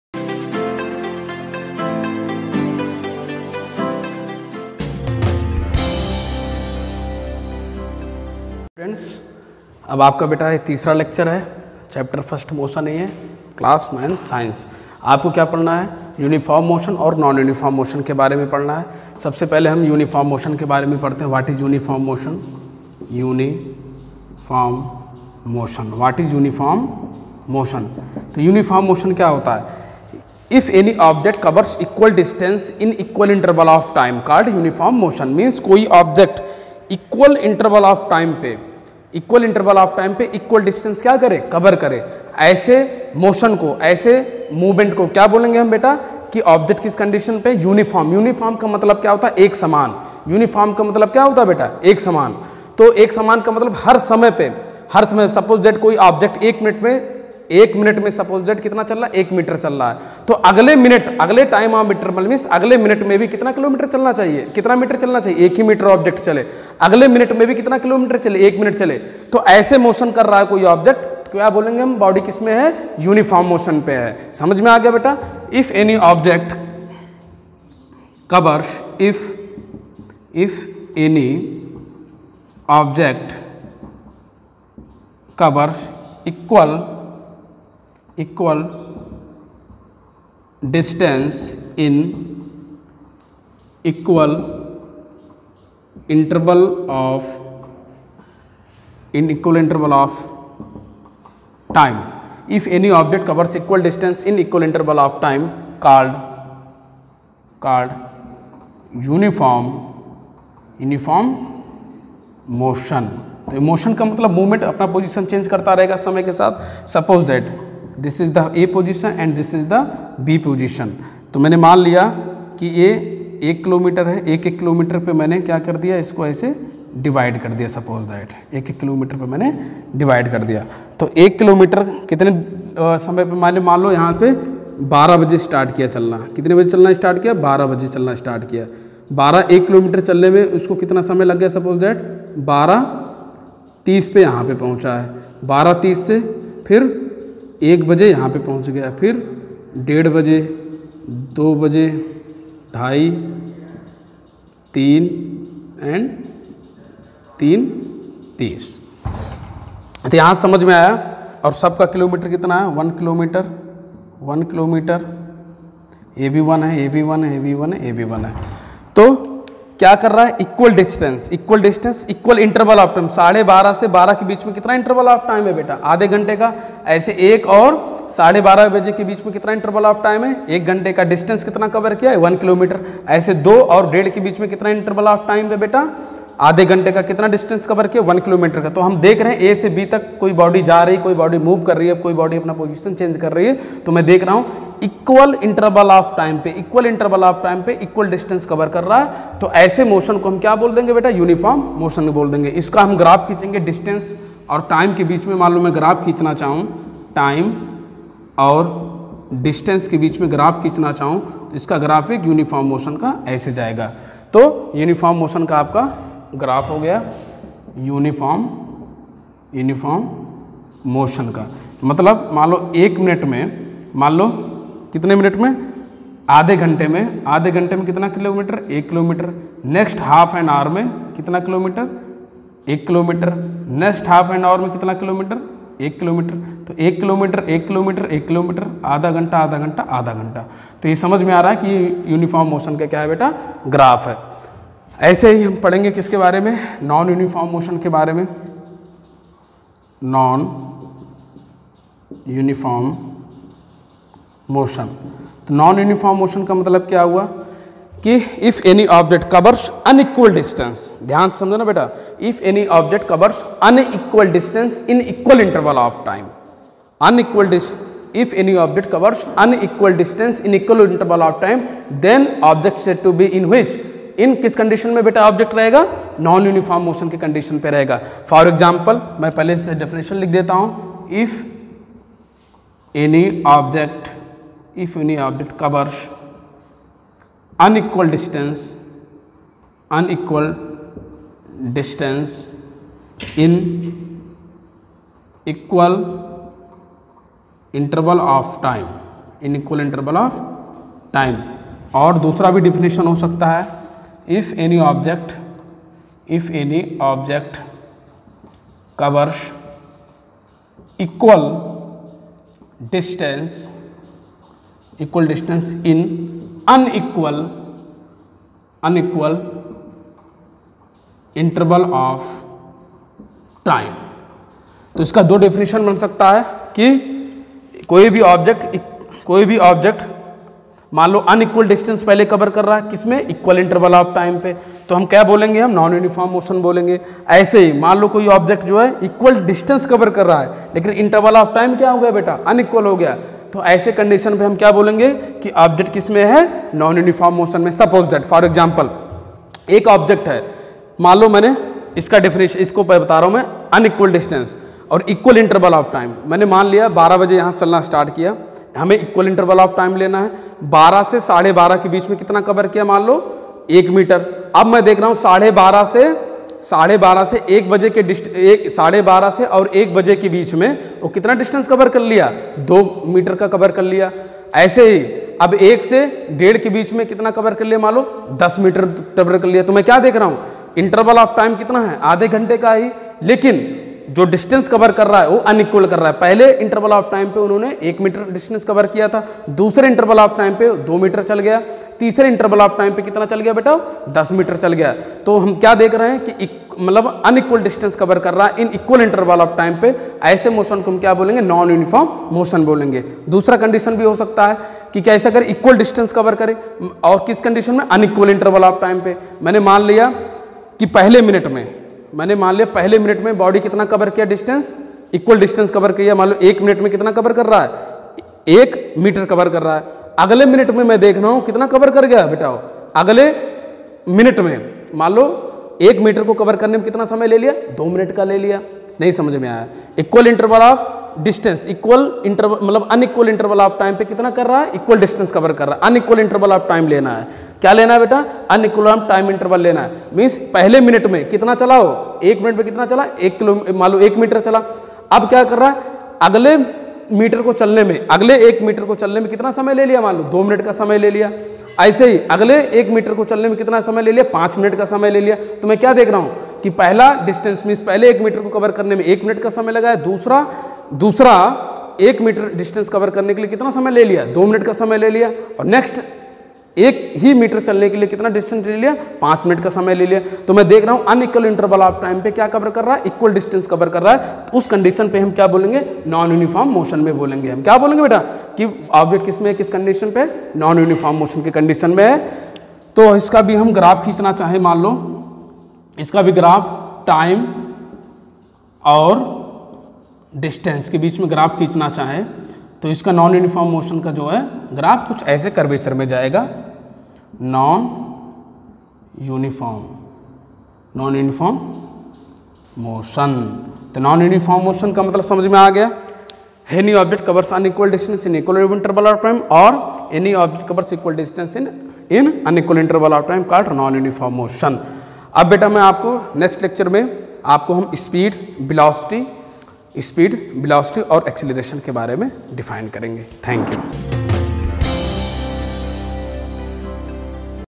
Online classes (Class 9th) NDM Sr. Secondary School Punhana Mewat)
9th Class - Science
Chapter 1 Motion
Lecuture -3 (Science)